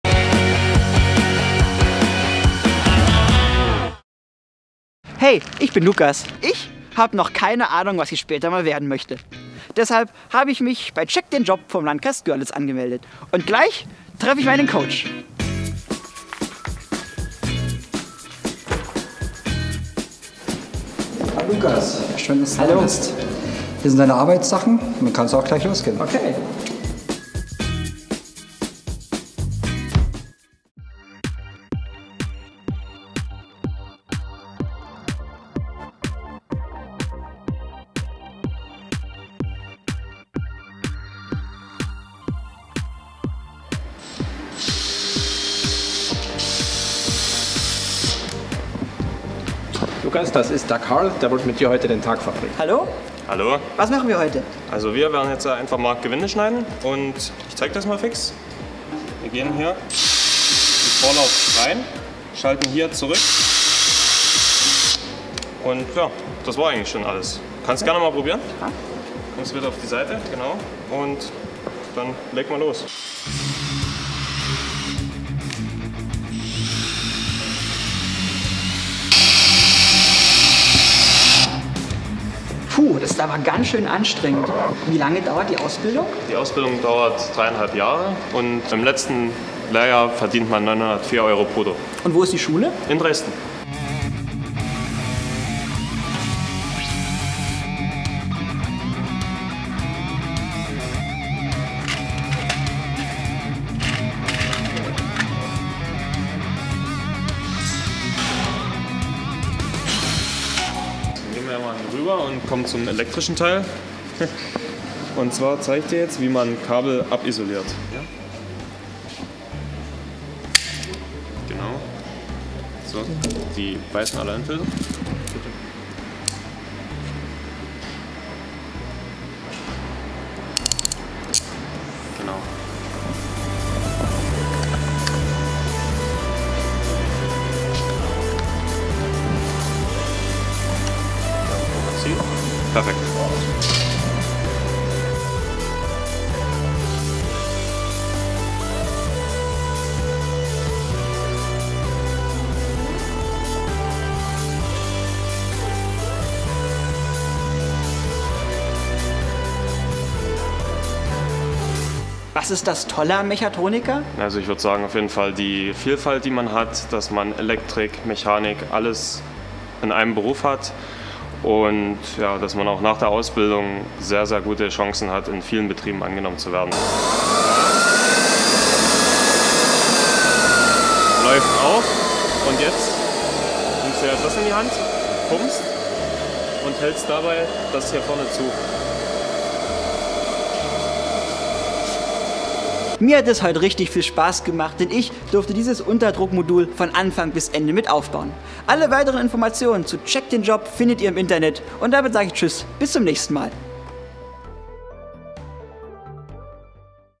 Mechatroniker | Lukas checkt den Job | Lukas vor Ort | mit Lukas Hässner
Ich probiere den Beruf des Mechatroniker einen Tag lang aus und werde dabei mit der Kamera begleitet.

Ich habe mit mechanischen, elektrischen und elektronischen Komponenten gearbeitet und diese zu einem Unterdruckmodul montiert.

Es hat mir sehr viel Spaß gemacht, weil ich so die Funktionsweise und den Aufbau von diesem Bauteil viel besser kennenlernen konnte.

[mit englischem Untertitel - white English subtitles]

Die wichtigsten Infos:
Gehalt im dritten Lehrjahr: 904,00 €
Ort der Berufsschule: Dresden
Ausbildungsdauer: 3,5 Jahre

Moderation: Lukas Hässner
Kamera und Postproduction: Franz-Ferdinand Kießling

Natürlich sind die die Gegebenheiten in jedem Unternehmen etwas anders. Wir haben dieses Video bei der ULT AG Umwelt-Lufttechnik gedreht.

Die Aktion CHECK DEN JOB ist ein Projekt vom Landkreis Görlitz.

Auch wenn wir dieses Video für den Landkreis Görlitz gedreht haben, ist diese eine eigenständige Arbeit von mir und meinem Team.